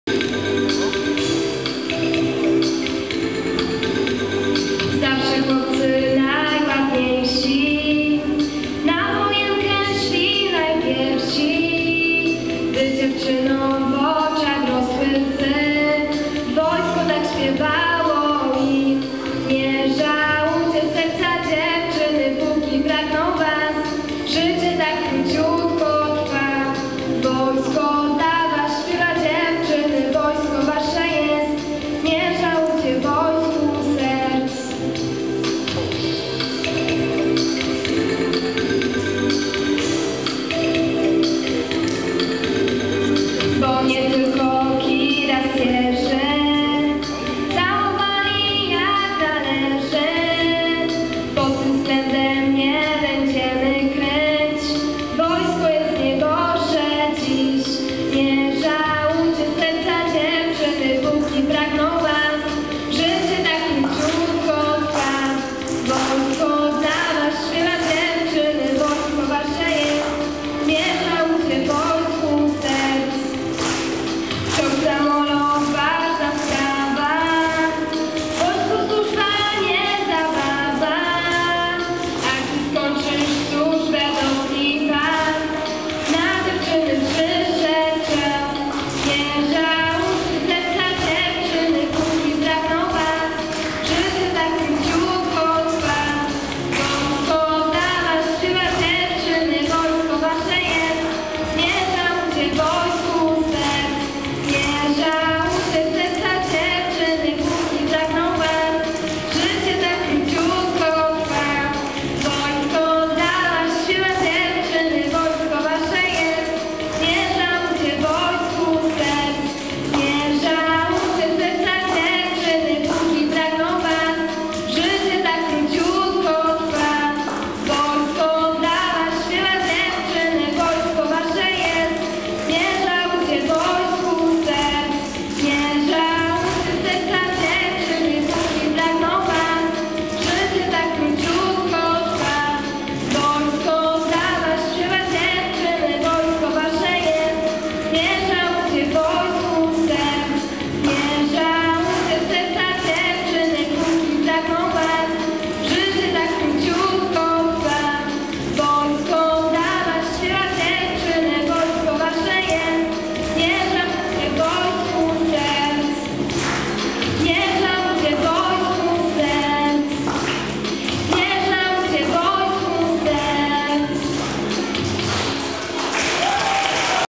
Akademia Gawłuszowice (2)
25-ta rocznica nadania imienia Władysława Jasińskiego "Jędrusia" Szkole Podstawowej w Gawłuszowicach - akademia(2)